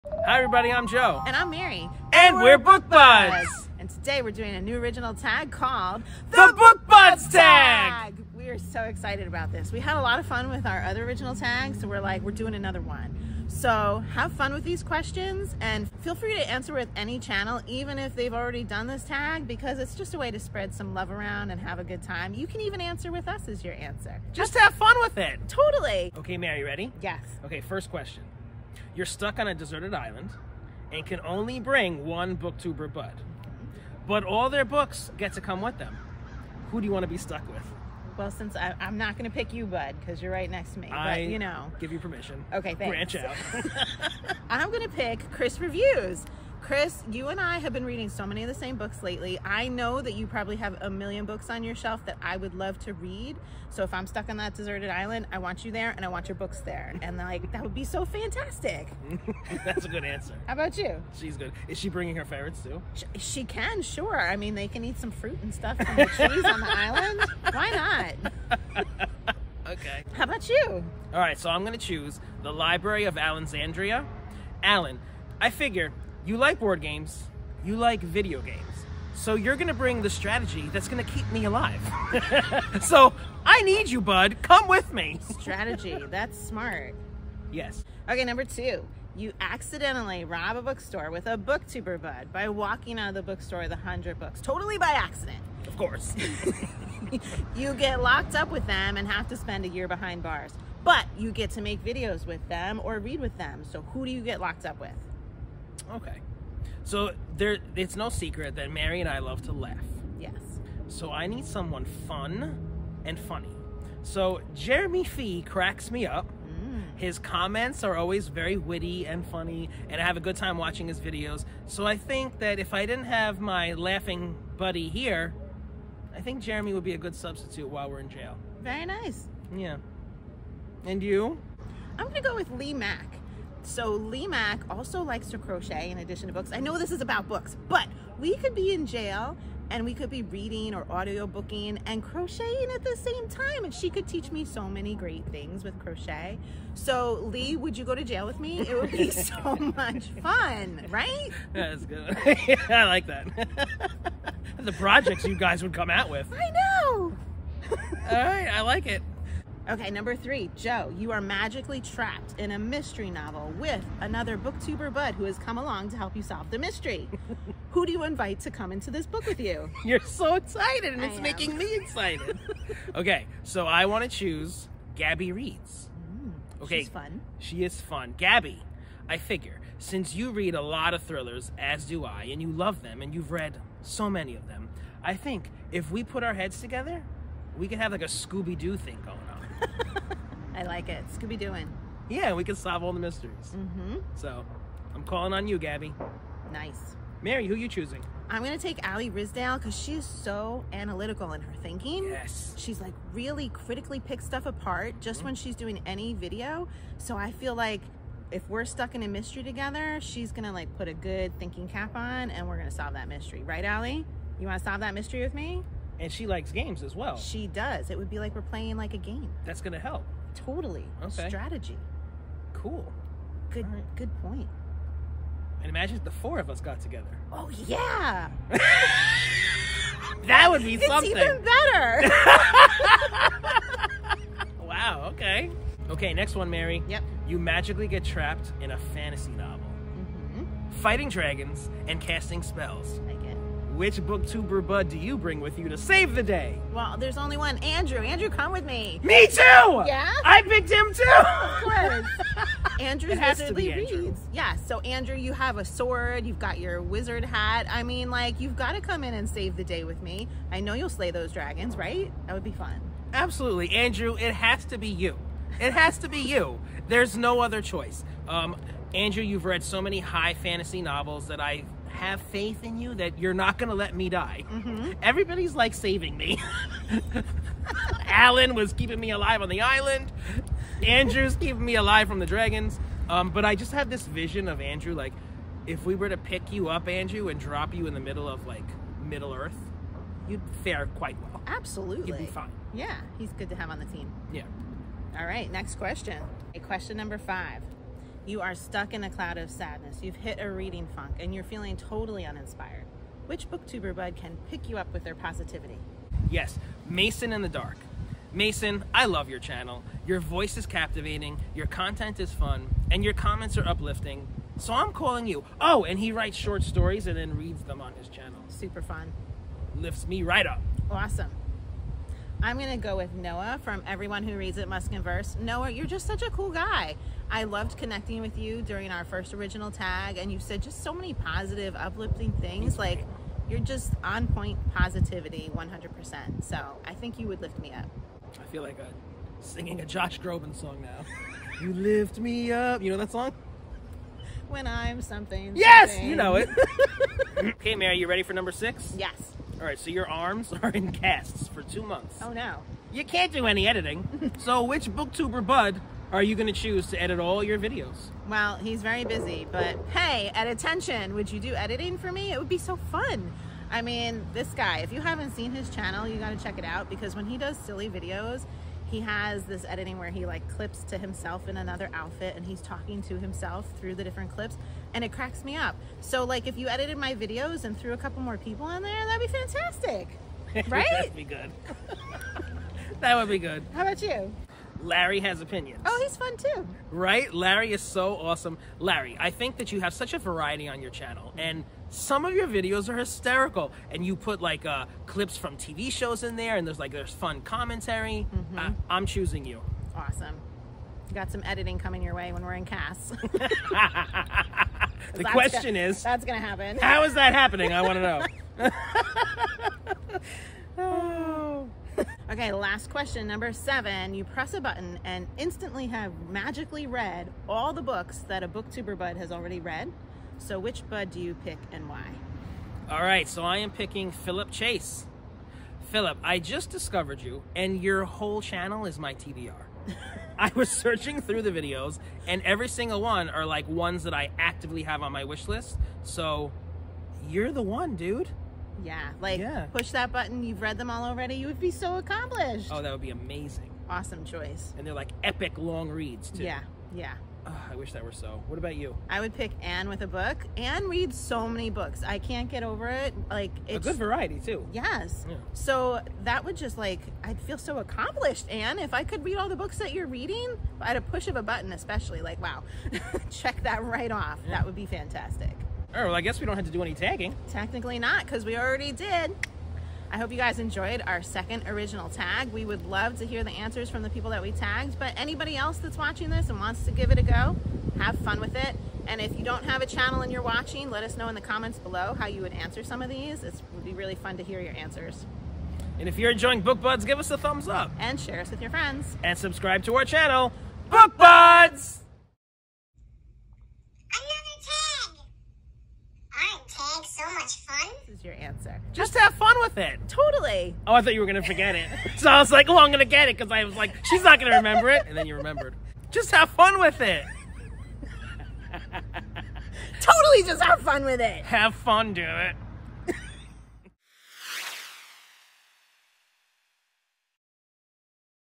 The Book Buds Tag! (Original Tag)
The Book Buds Tag! (Original)
Our 2nd Original Tag!! So excited!

We’re ALL Book Buds here on booktube- so let’s have some fun with our buds and see who has the strength of character to help us out in some unusual circumstances!

If you do this tag- please comment here so we can come check it out!

Questions & Timestamps

(0:00) Intro to The Book Buds Tag!
(0:27) 1. You’re stuck on a deserted island and can only bring one booktuber bud, but ALL their books would come with them - who do you want to be stuck with?
(1:28) 2. You accidentally rob a bookstore with a booktuber bud by walking out with 100 books.
You spend a year behind bars with them, BUT now you can read and do videos together - who do you get locked up with?
(2:41) 3. You magically get trapped in a murder mystery you are reading. Another booktuber bud joins you to solve the mystery! – Who do you want to join you?
(3:56) 4. You magically get trapped in a fantasy novel fighting dragons and casting spells. Which booktuber bud do you choose to bring in with you to save the day?
(4:56) 5. You are stuck in the cloud of sadness, hitting a reading funk, and feeling totally uninspired. Which booktuber bud can pick you up with their positivity?
(5:49) 6. Your arms are in cast for two months and a booktuber bud is going to edit all your videos for you. - Who edits them and why?
(6:52) 7. You press a button and instantly have read ALL the books that a booktuber bud has already read. - Which bud would you pick and why?
Remember- you can answer with people who have already done the tag, even with the original Book Buds creators! Just have fun with it! And no need to tag additional people- because you basically just did! Let YOUR Book Buds know you mentioned them and let’s see what they do with this tag!

As Always… Bloopers/Outtakes! (8:18)